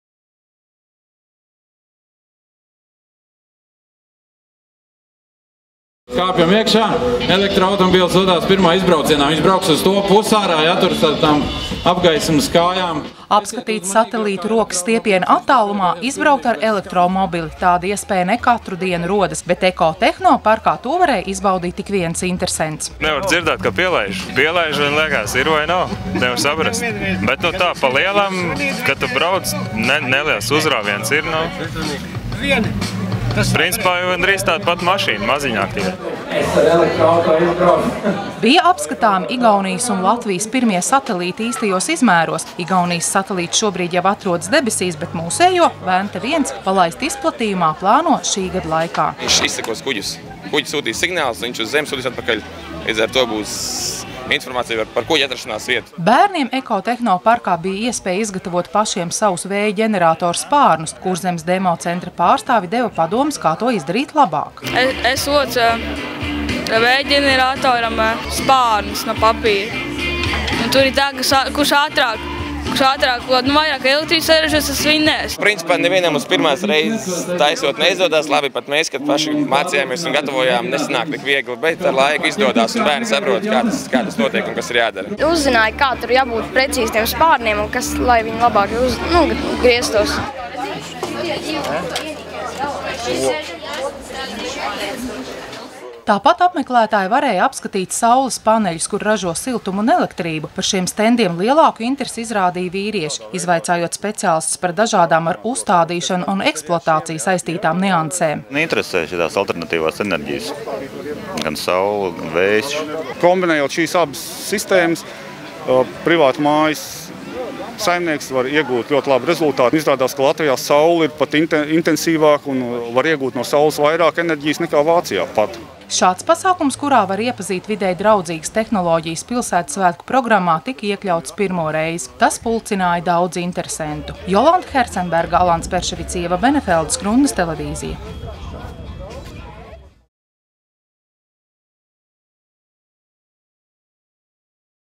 Dzīres Kuldīgā. Eko - tehno parks
Kuldīgas Pilsētas svētkos pirmo reizi apmeklētājiem tika vērts Eko - tehno parks. Tajā bija iespēja aplūkot Latvijas un Igaunijas pirmos satelītus, interesenti varēja doties testa braucienā ar elektromobili. Pie saules paneļiem pulcējās vīri, kuriem interesēja praktiski jautājumi, kas saistīti ar to uzstādīšanu un izmaksām, bet bērni gatavoja paši savus vēja ģeneratora spārnus.